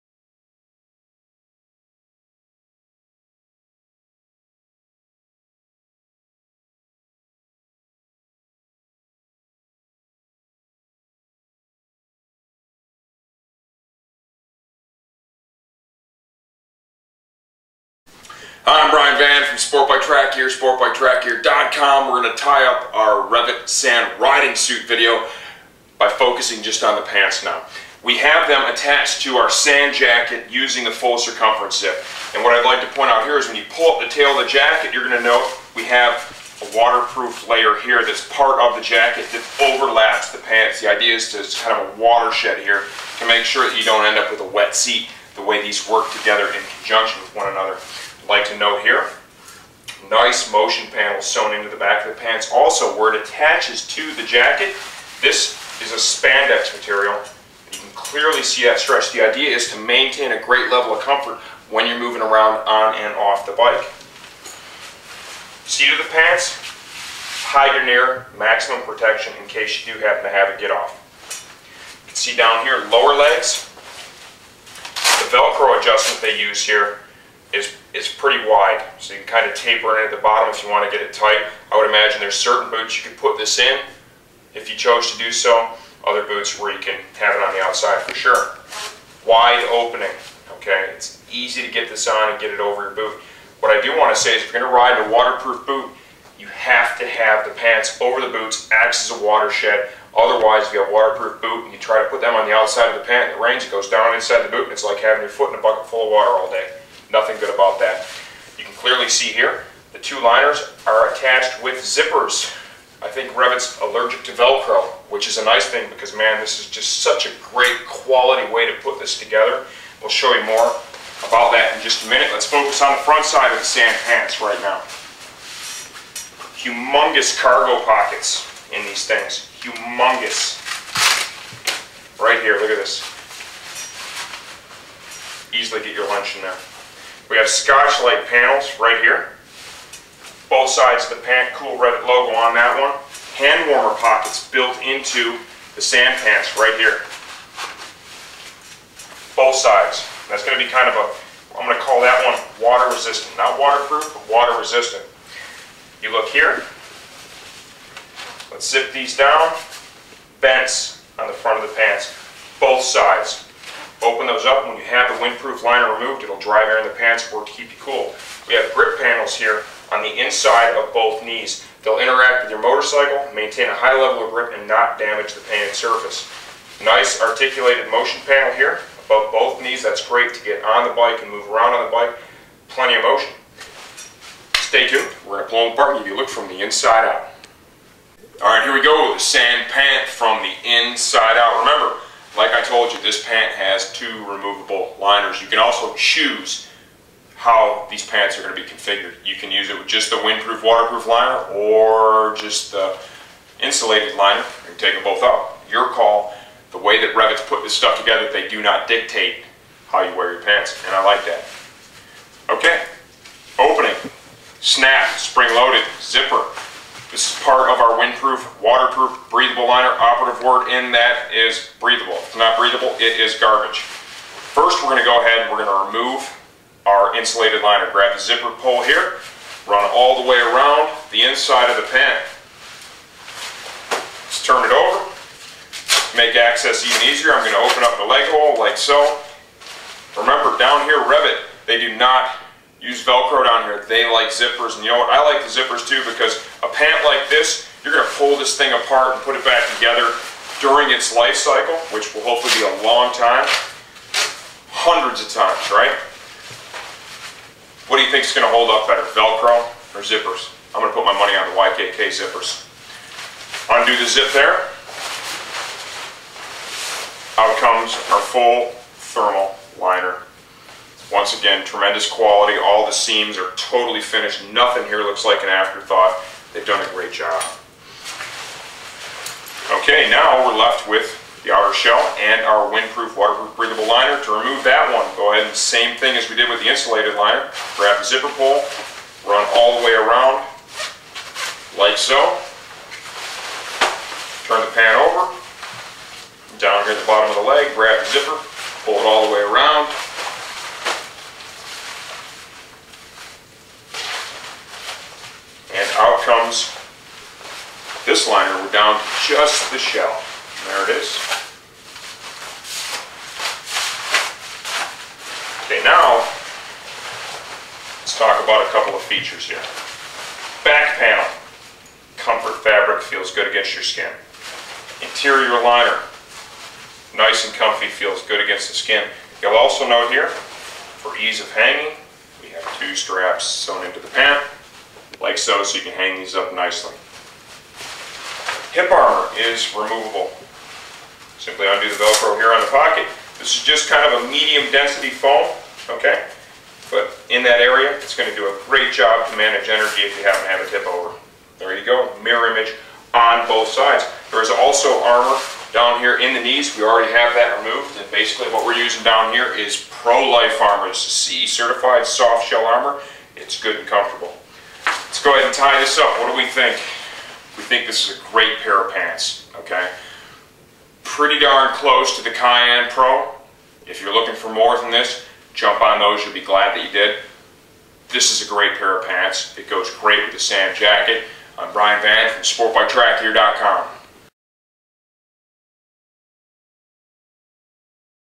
REV'IT! Sand Pants Video Review from SportbikeTrackGear.com
We cover features, benefits and sizing for The REV'IT! Sand Pants.